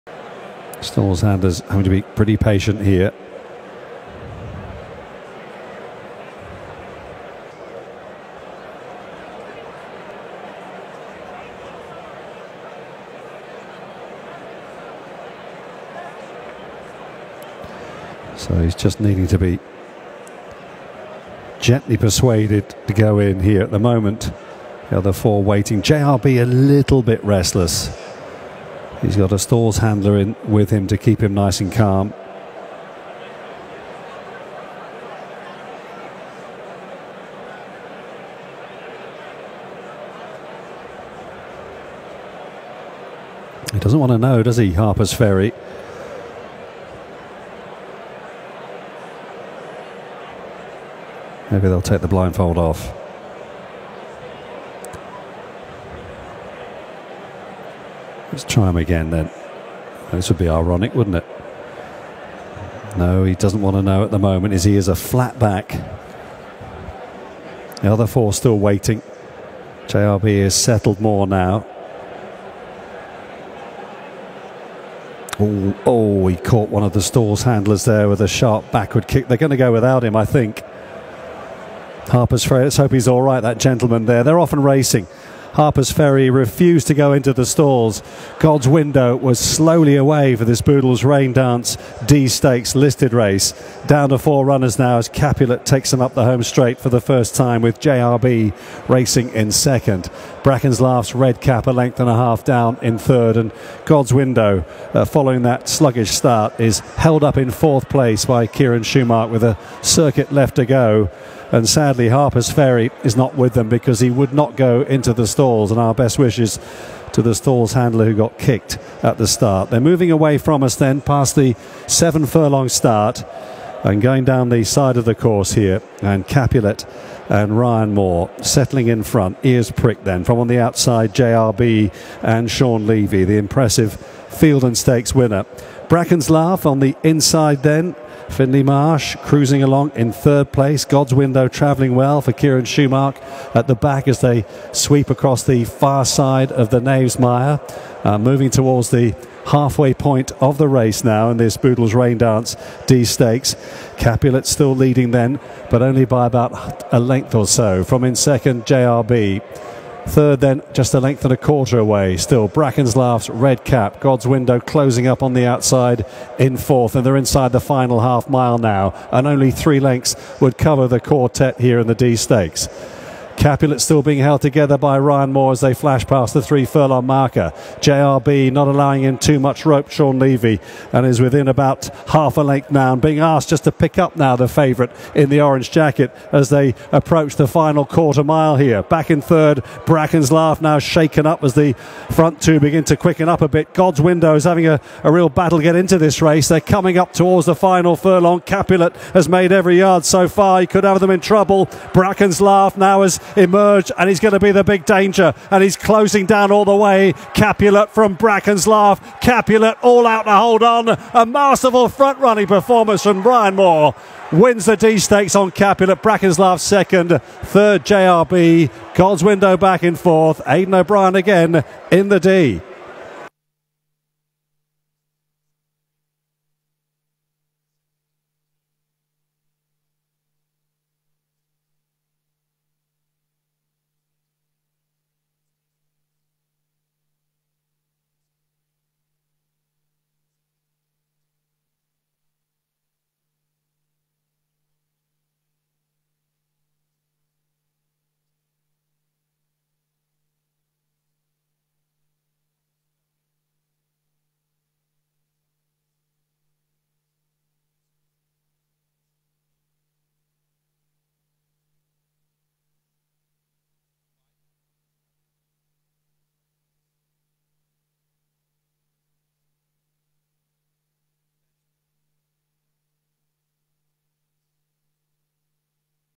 Race (4) Stall Handler gets a kick from Harpers Ferry I hope he is ok. 15:05 Raindance Dee Stakes
Race (4) 15:05 Chester 09 May 2024 Sky Sports Racing
Boodles Raindance Dee Stakes

Listed | Class 1 | 3YO only

Winner £56,020 - 5 run

1st  (3) Capulet 7/2
Winning jockey: R L Moore Winning trainer: A P O'BrienRunners:
All 4 ran Withdrawn: (5 F: 3rd (1) Jayarebe 7/4 Off Time: 15:07:42
CSF £15.62 (3, 2)